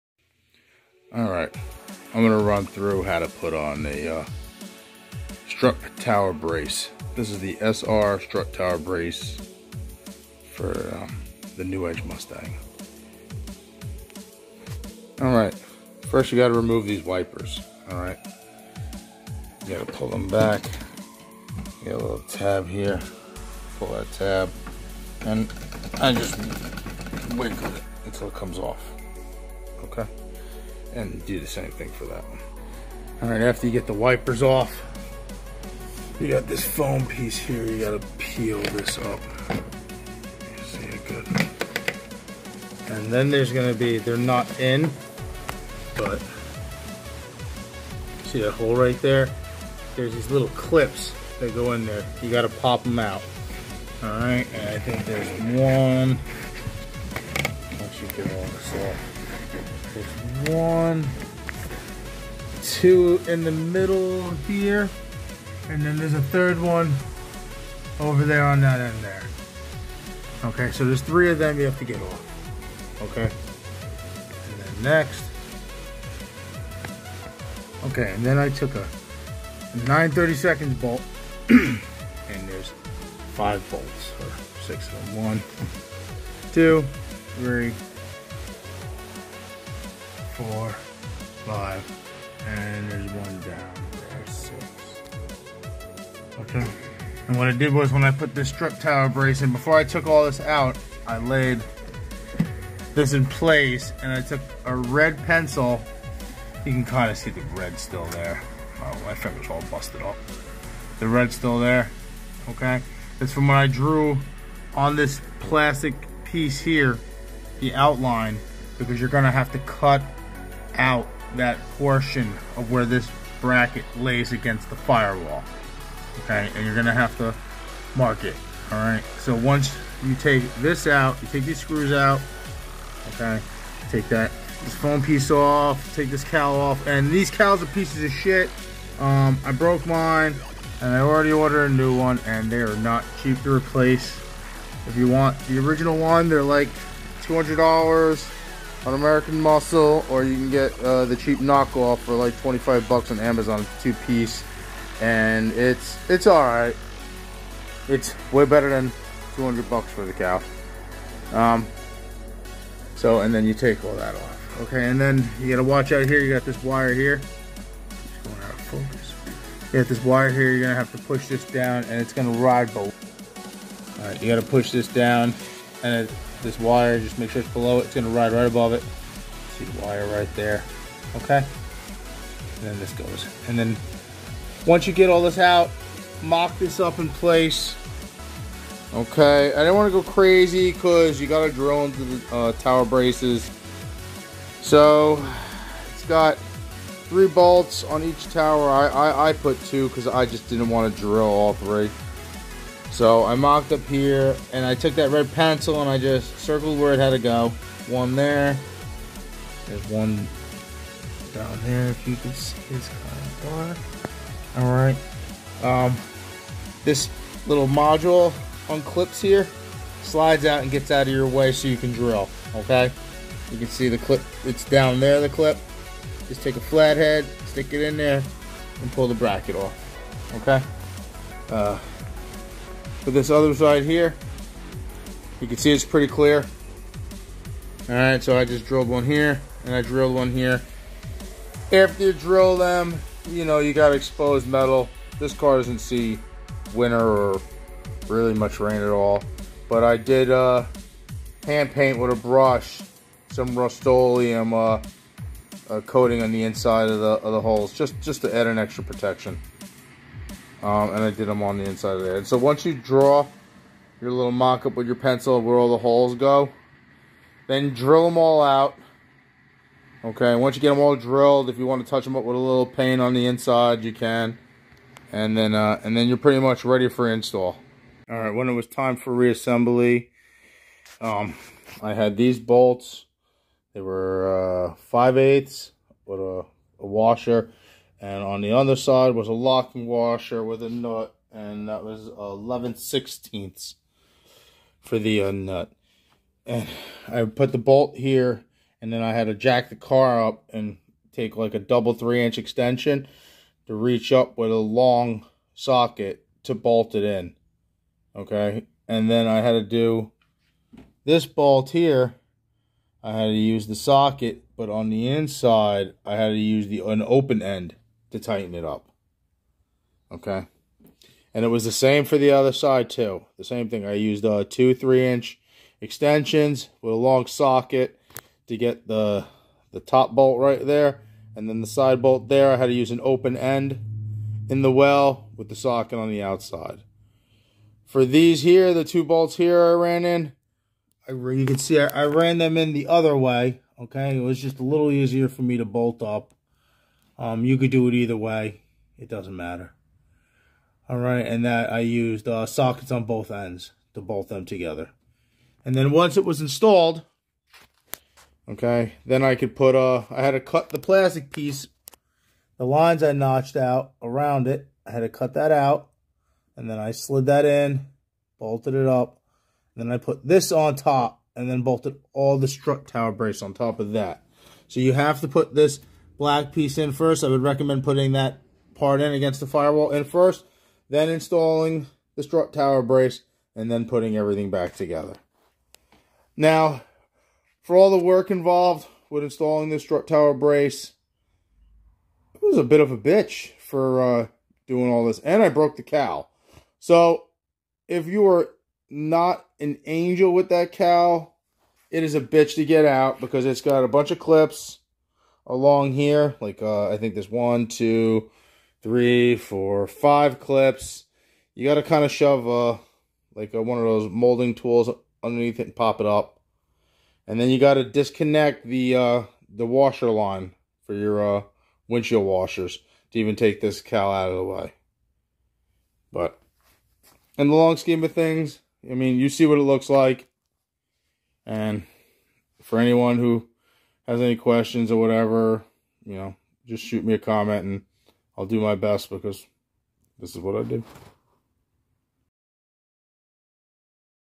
Ford Mustang strut tower brace install 1999-2004 GT and V6
you got to cut drill and most likely break your $$ wiper cowl to put this on. (sr strut tower brace )